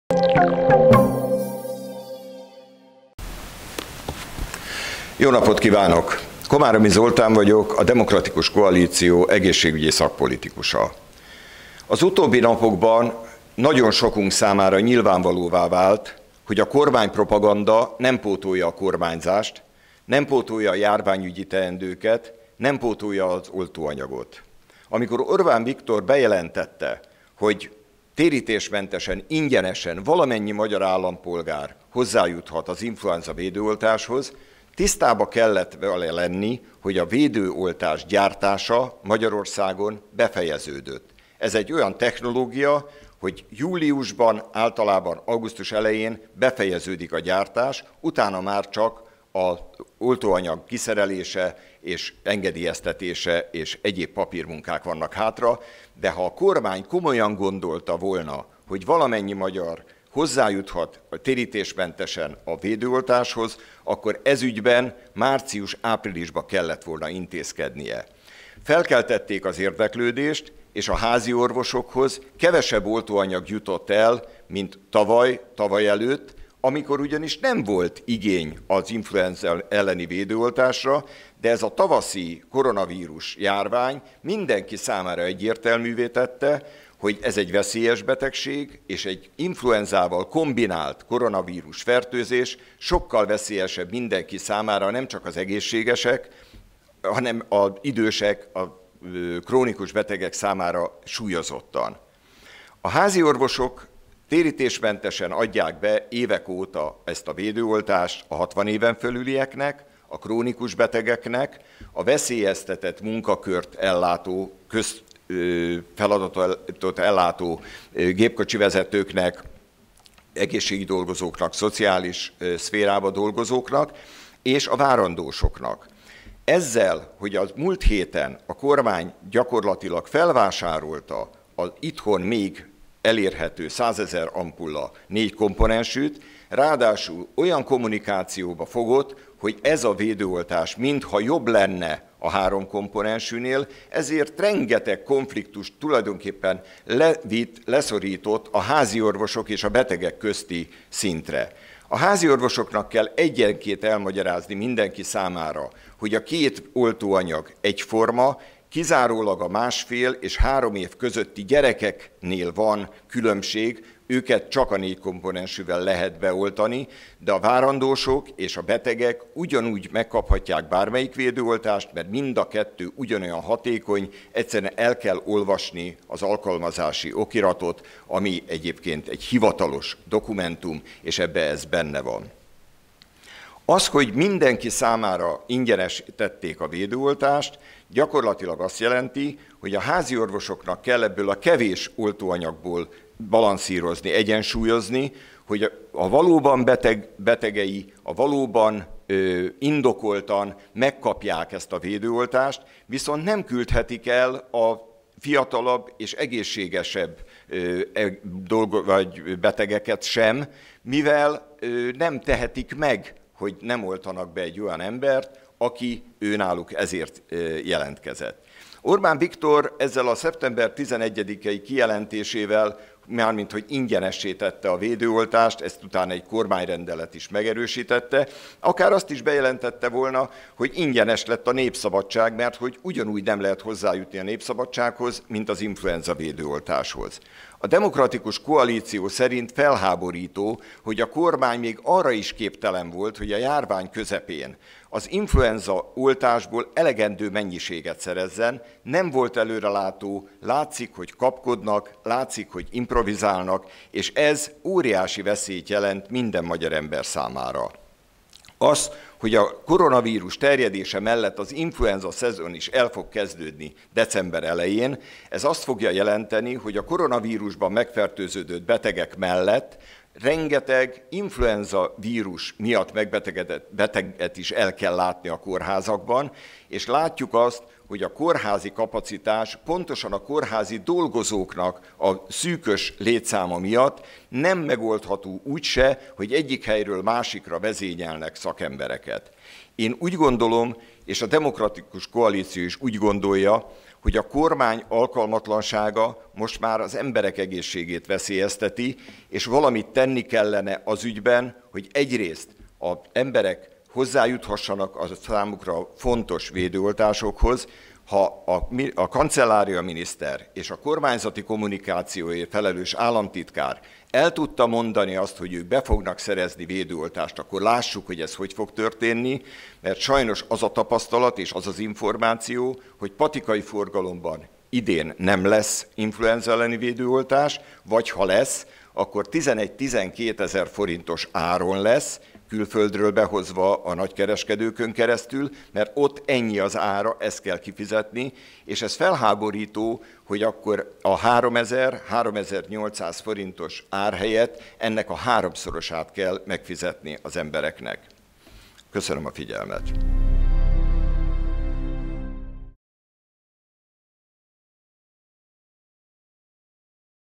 Nincs elég influenza elleni védőoltás!
Felháborító, hogy a kormány nem volt képes elegendő influenzaoltást beszerezni a járvány kellős közepén.
A kormány hibája miatt influenzás betegek fognak a covid-osztályokra kerülni!

Oszd meg Dr. Komáromi Zoltán online sajtótájékoztatóját!